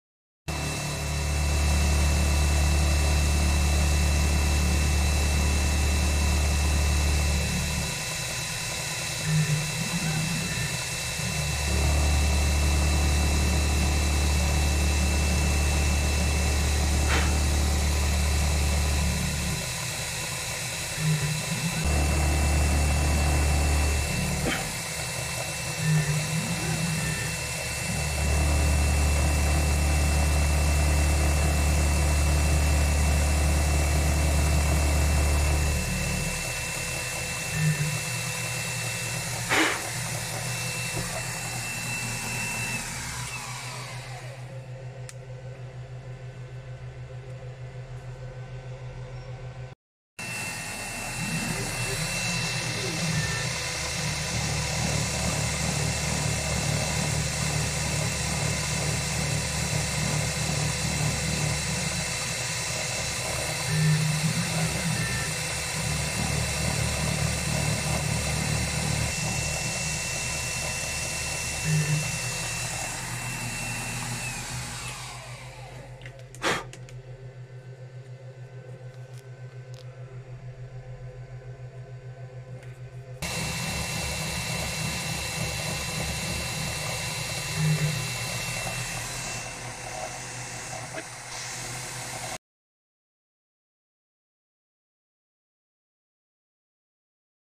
Knurling on the 4th axis
It's not fast, but it works pretty well.